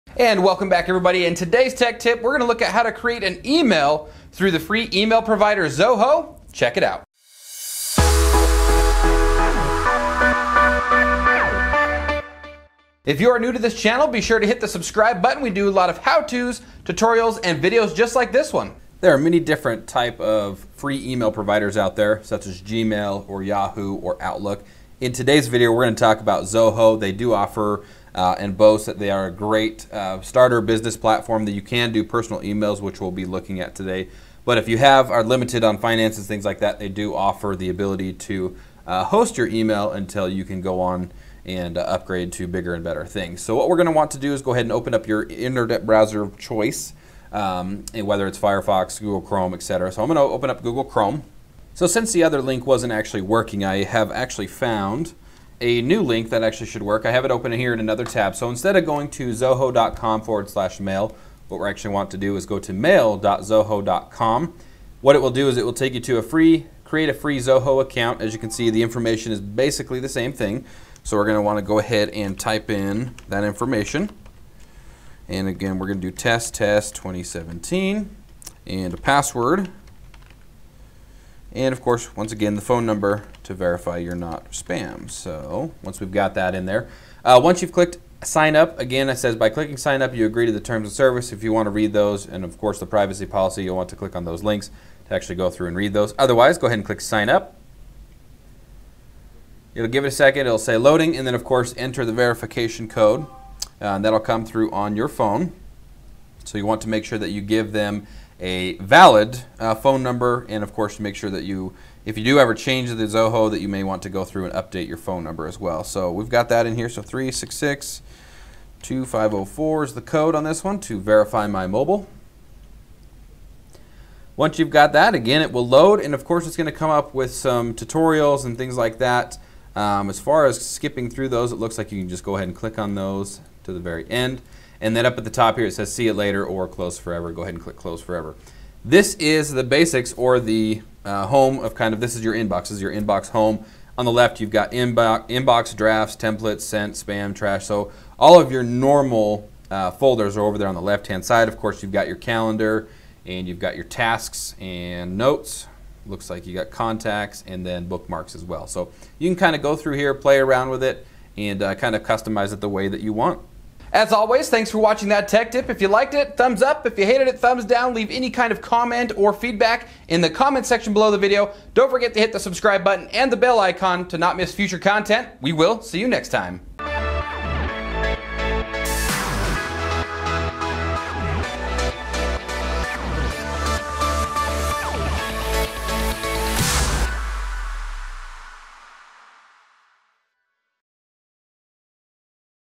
How to Create a Zoho Email Account
There are a ton of free email providers out there. ZOHO is no stranger. They are a great option for new or existing businesses. In this video, we help you create a free personal account.

HelpCloud’s Filming Equipment List